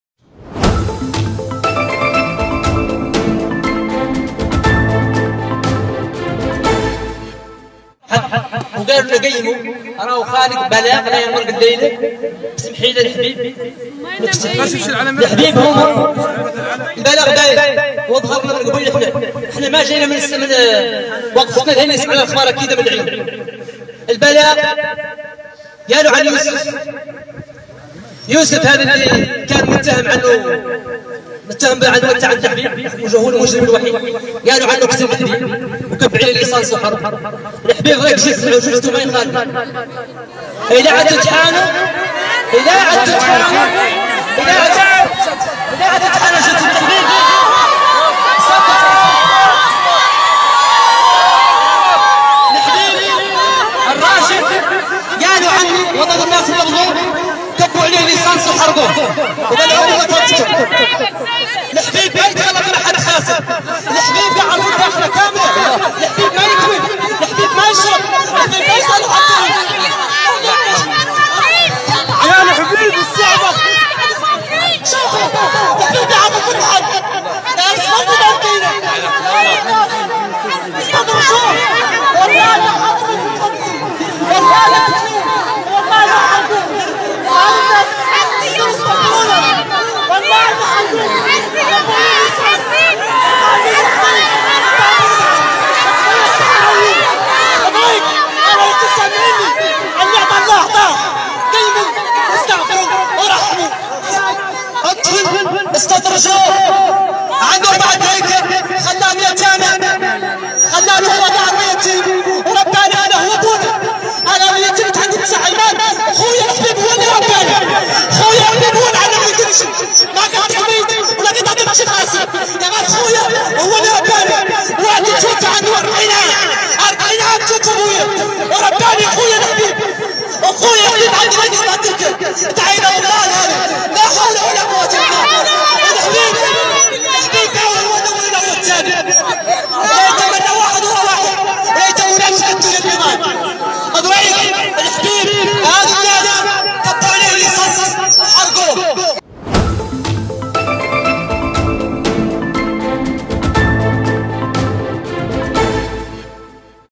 شقيق لحبيب اغريشي يطالب بجثته ويؤكد أنه قتل حرقا
ليلة بيضاء بطابع الحزن والاسى قضتها أسرة لحبيب اغريشي، المواطن الذي أعلن عن اختفائه منذ 10 أيام بالداخلة.
وتظاهرت أسرة لحبيب والعشرات من ساكنة الداخلة أمام مفوضية الأمن مساء الأربعاء 16 فبراير 2022، حتى الساعات الأولى من صبيحة اليوم الموالي، مطالبين بالكشف عن كامل الحقيقة حول مصير ابنهم رجل الأعمال، خصوصا بعد العثور على الشاهد الوحيد في القضية ميتا بعد ساعات من انتهاء التحقيق معه، في مكان قرب الشاطئ.

# أخبار_عاجلة